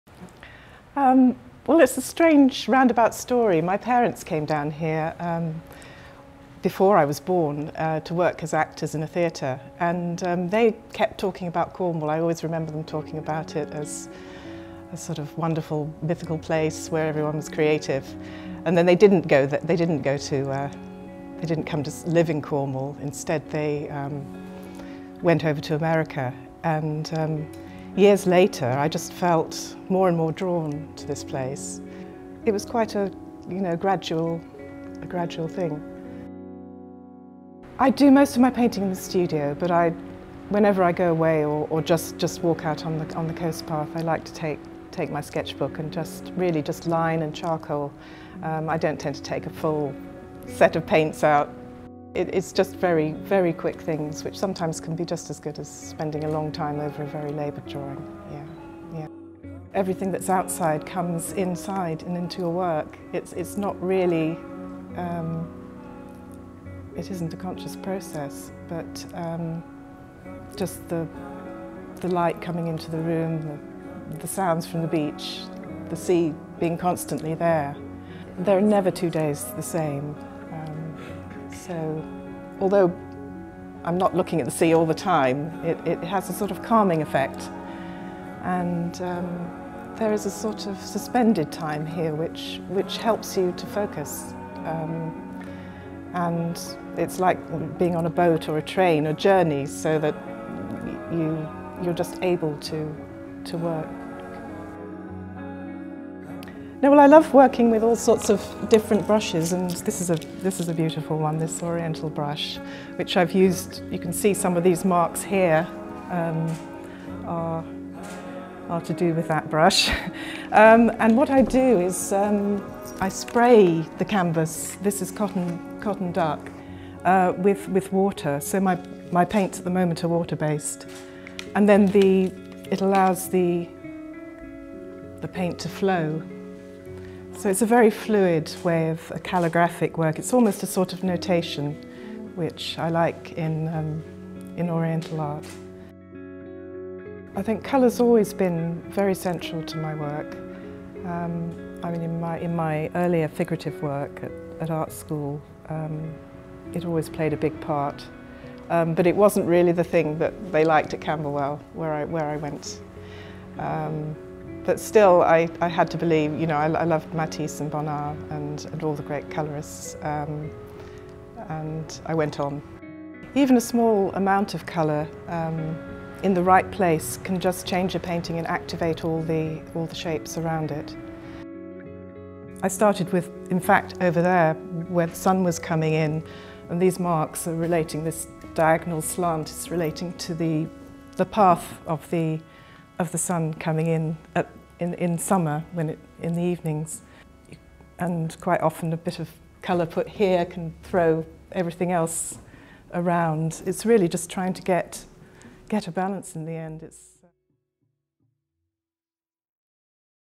Seasalt Cornwall: Modern Creatives Felicity Mara
We spent the day at Porthmeor Studios with artist Felicity Mara, our latest Modern Creative, to discuss what drew her to Cornwall and why she prefers her paintings to be a bit mysterious...